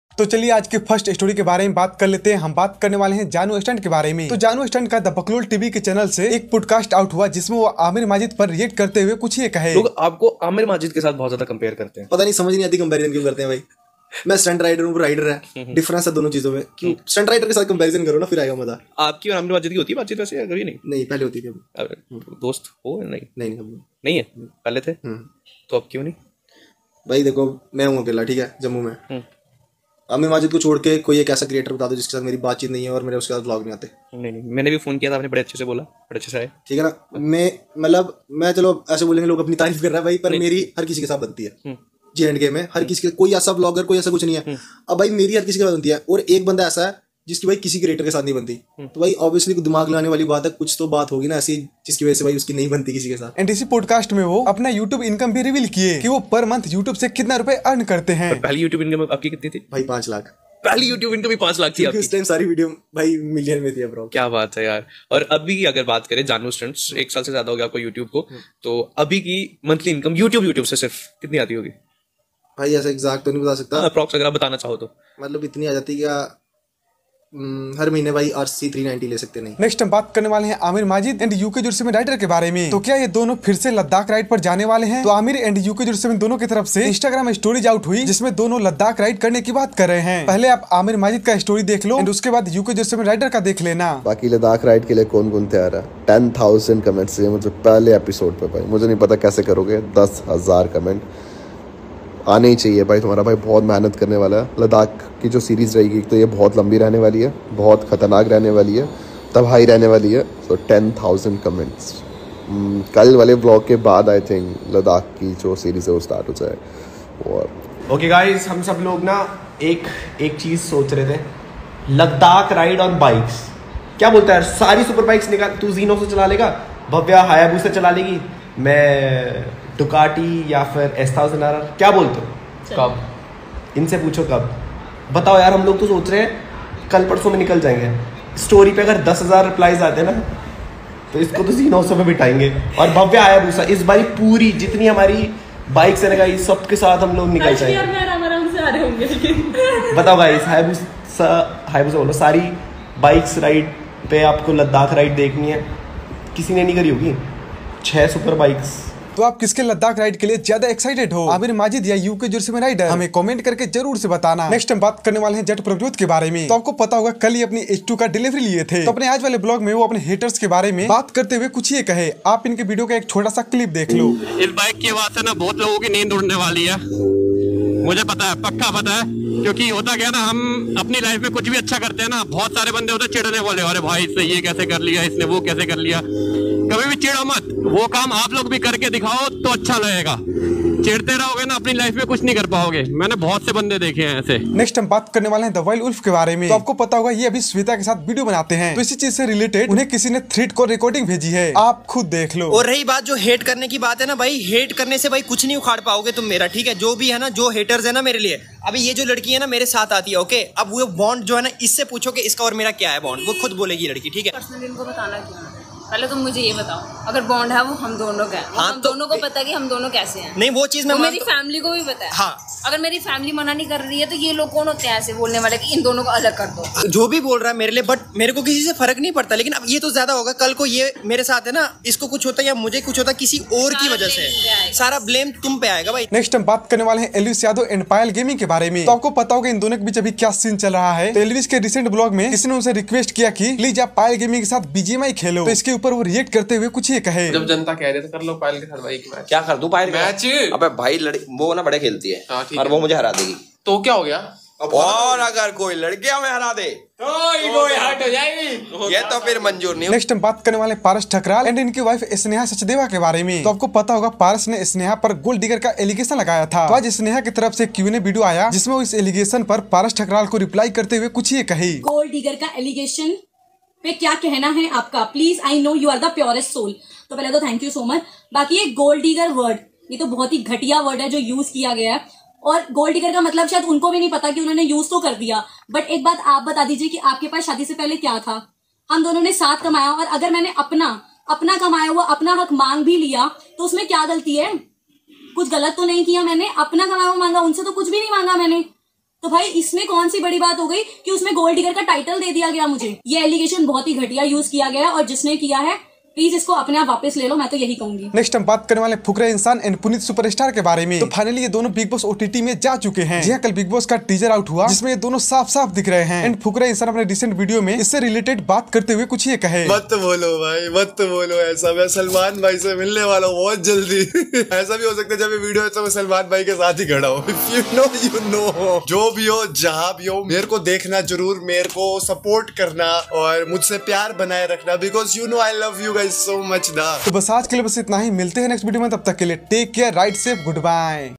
& The Uk07 Rider Both On Ladakh Ride, Elvish Yadav React, proper training workout
Jannu
Jannu Stuntz
Jannu Stuntz Vlogs
Moto Vlogger Jannu Stuntz

Topic include:- First- Jannu Stuntz POKED Aamir Majid
Next-Jannu Stuntz YouTube Income REVEAL
Next Aamir And The Uk07 Rider Both Going For Ladakh Ride
Next- Jatt Prabhjot Reply To Haters
Next- Teh The Wild Wolf Reply To Haters
Next-Elvish Yadav And Payal Gaming
Next-Sneha Sachdeva Reply To Paras
Next- Fukra Insaan And Punit Superstar In Biggboss Confirm